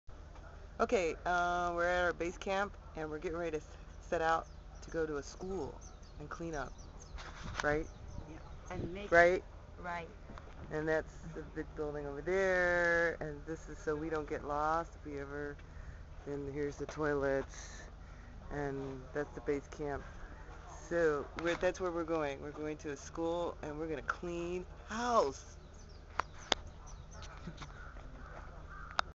Magic Bus: Miyagi Prefecture June 2011
Video documentation of "The Magic Bus Project". June 2011, 5 people from Tokyo go to volunteer in Miyagi prefecture as part of a project called the "Magic Bus" created by Toshi Homma.